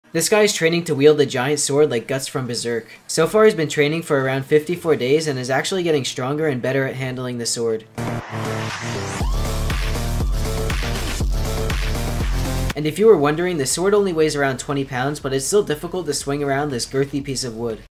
Giant Sword Training
This guy is training to wield a giant sword